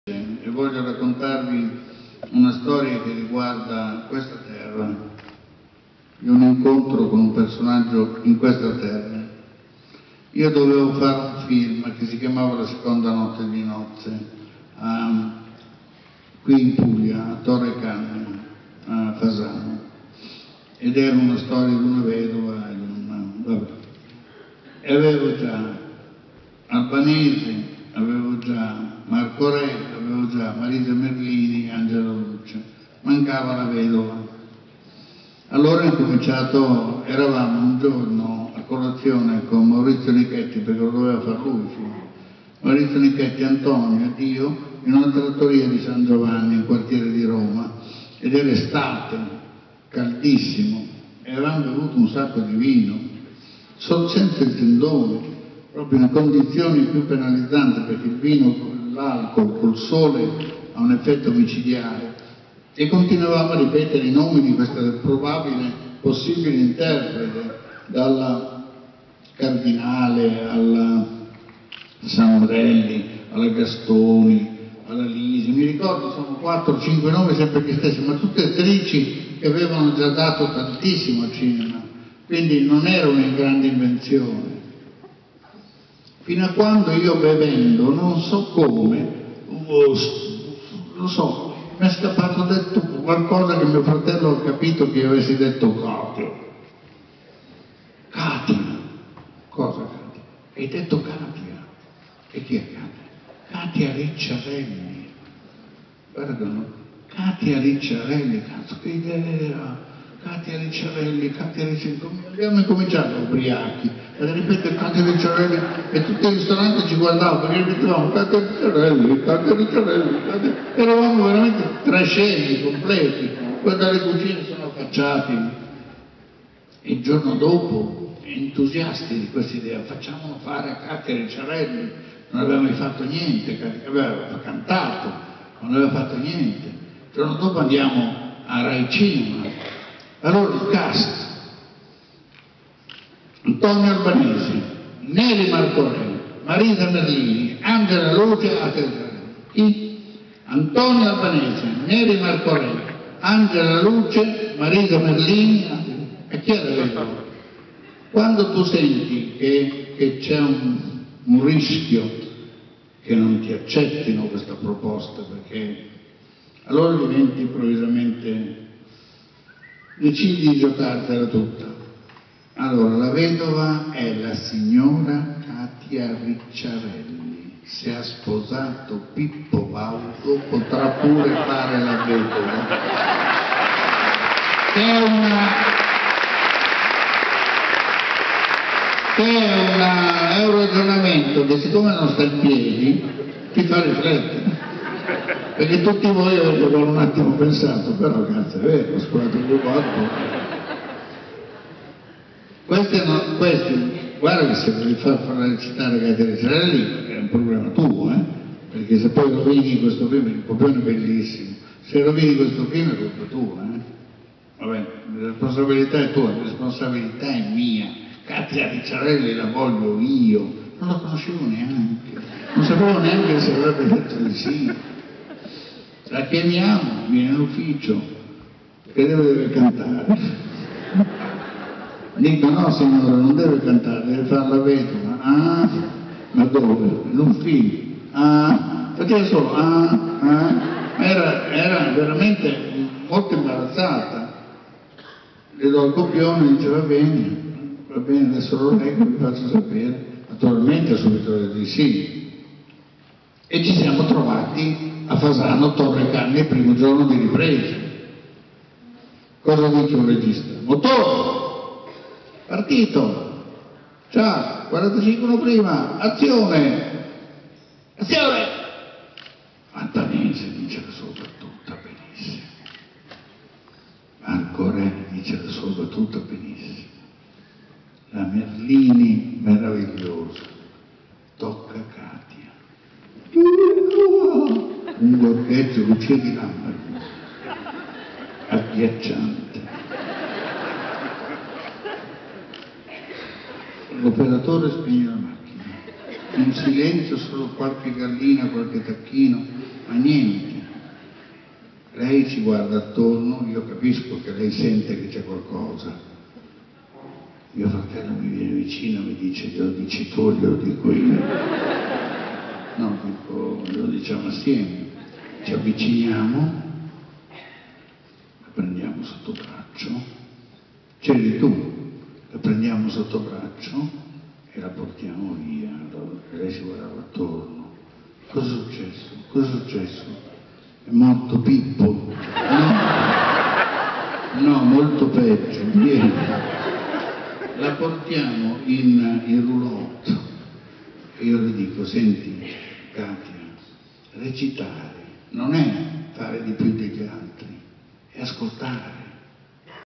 BIF&ST 2017 - Pupi Avati: La seconda notte di nozze e Katia Ricciarelli
- In questo video Pupi Avati, al BIF&ST 2017 per una masterclass a lui dedicata, racconta invece il rapporto con Katia Ricciarelli, alla sua prima esperienza come attrice sul set de La seconda notte di nozze. La scelta fu rischiosa ma il risultato al di sopra di ogni aspettativa